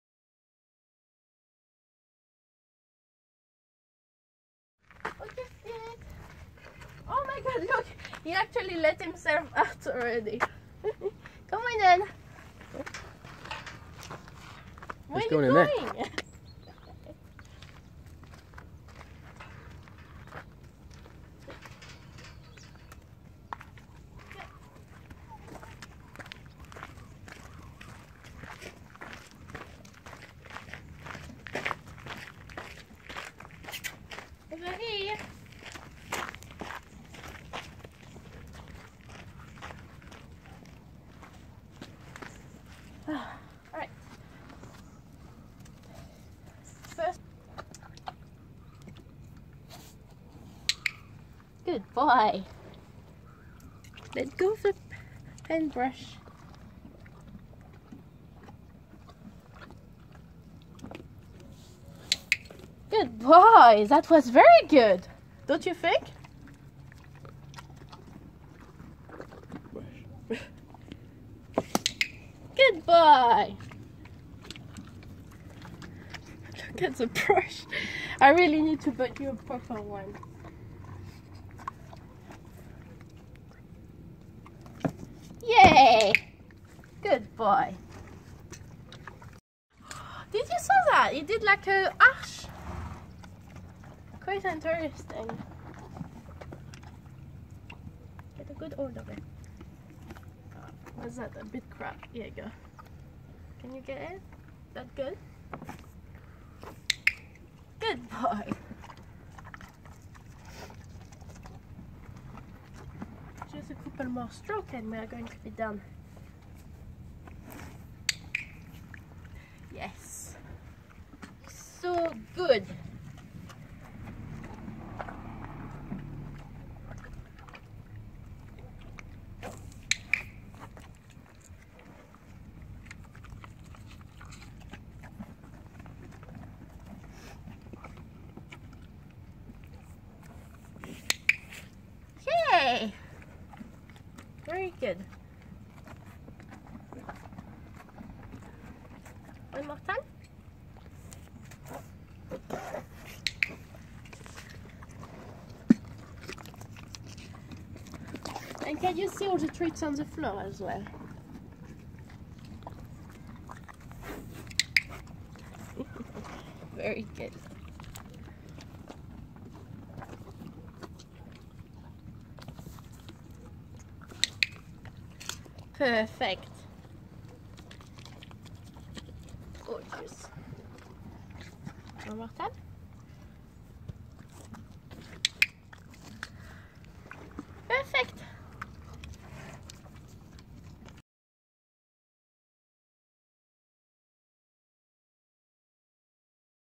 Painting by ponies: an interspecies collaboration
Pony Spirit paints in profit of equine charity, Prince Fluffy Kareem. To check out his paintings visit our facebook